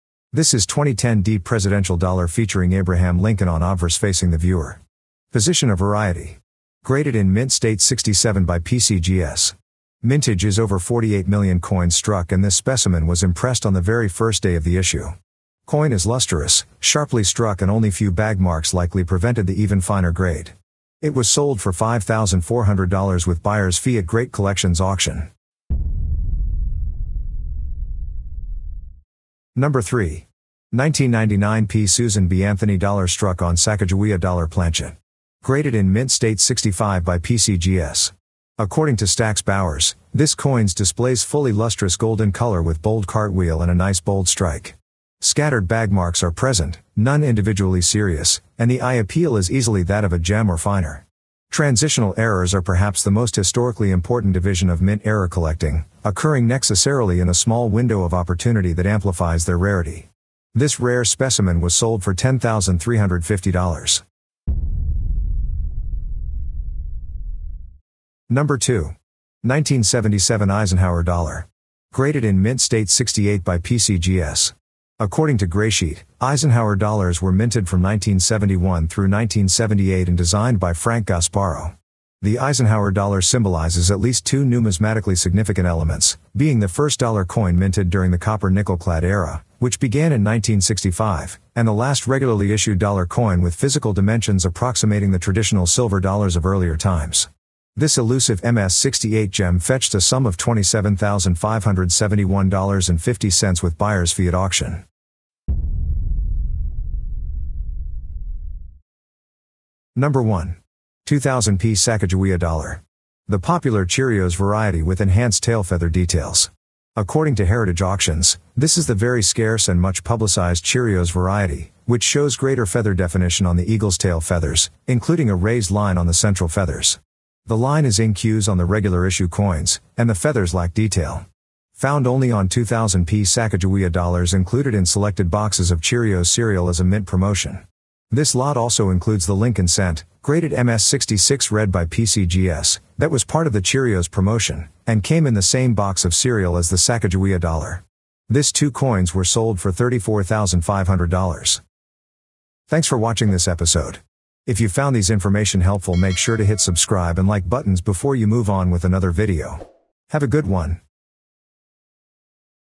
Rare Dollar Coins Worth Big Money
Discover rare dollar coins worth big money in this video! From silver dollars to valuable collector's items, learn about the most valuable dollar coins to look out for in your collection.